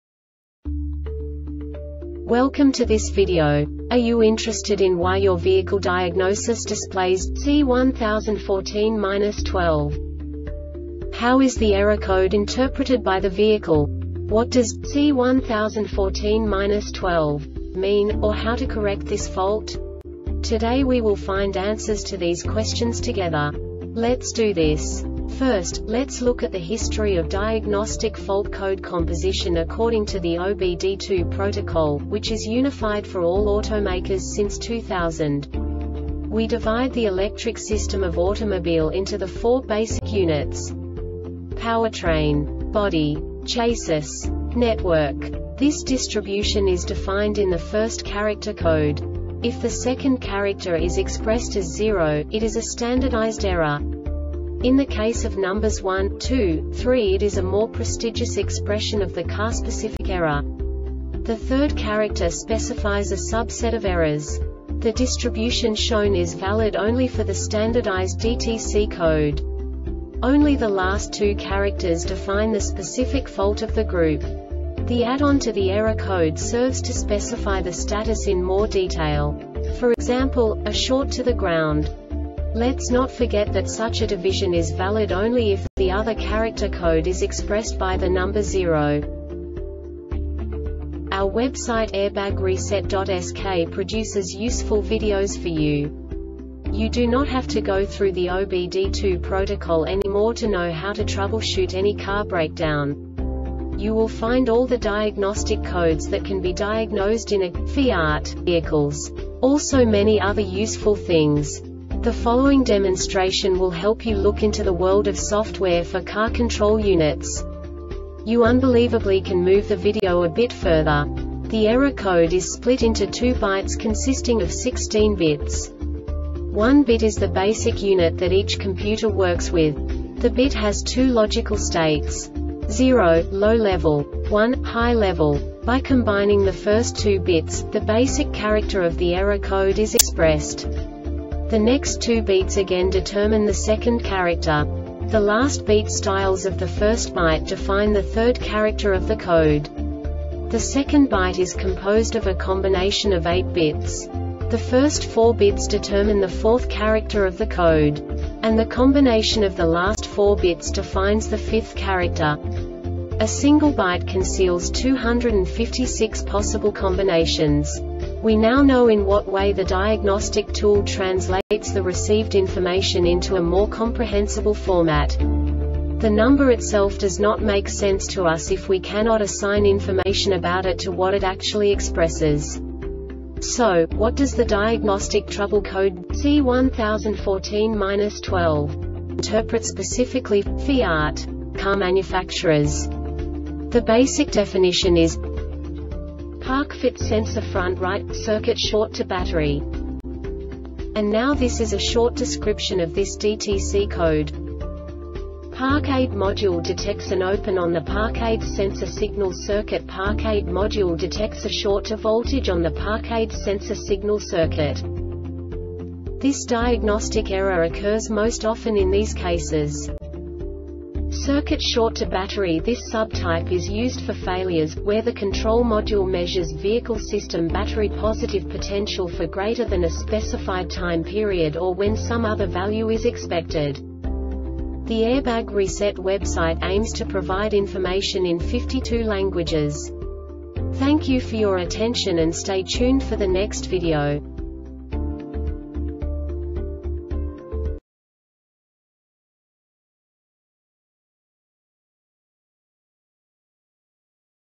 DTC Fiat C1014-12 Short Explanation
Contents:
0:21 Basic DTC analysis according to OBD2 protocol standard.
1:48 Insight into programming
2:58 A diagnostically understandable description of the Diagnostic Trouble Code
3:05 Basic error definition

Make:
FIAT
Code:
C1014-12
Definition:
Parkfit Sensor Front Right - Circuit Short To Battery
Description:
Park Aid Module detects an open on the Park Aid Sensor Signal circuit. Park Aid Module detects a short to voltage on the Park Aid Sensor Signal circuit.